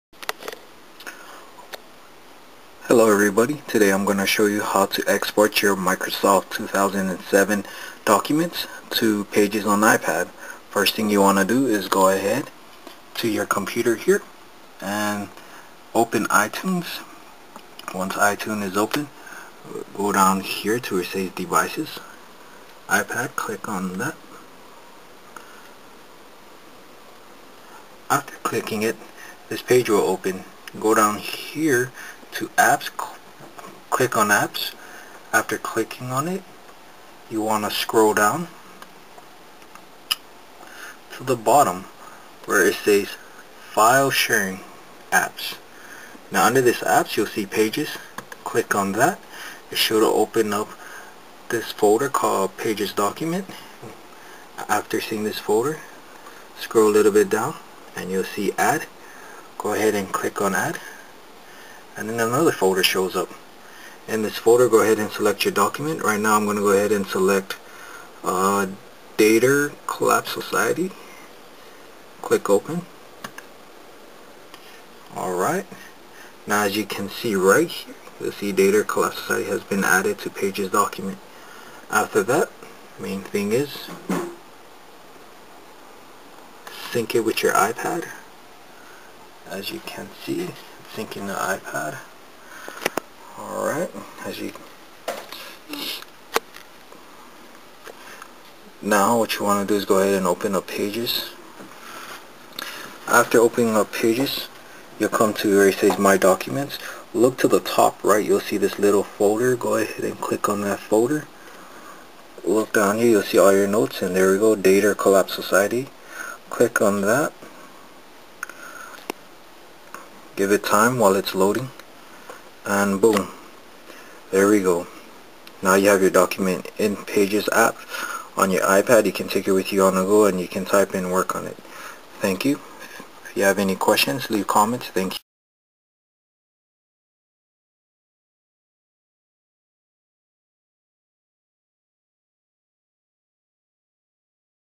How to transfer microsoft word documents to ipad pages app
How to transfer your microsoft documents to ipad pages app. This video is for those who are new to the ipad. If your a veteran just ignore this video. You can watch the video, or just read the tutorial here. Thanks!
Tutorial:
1. Open iTunes
2. Click on iPad under devices on left side of screen.
3. click of app tab on right side of ipad pic.
4. scroll down to file sharing app. click on pages icon.
5. click on add button on right side under pages document.
6. select your microsoft doc and click open.
7. click sync button in itunes.
8. open pages app on ipad.
9. in "My Documents" screen, click on folder at TOP RIGHT SIDE.
10. Select your document you uploaded.
Keep in mind that pages might not have many of the fonts found on microsoft word 07. thanks, plz comment and rate. have a great day.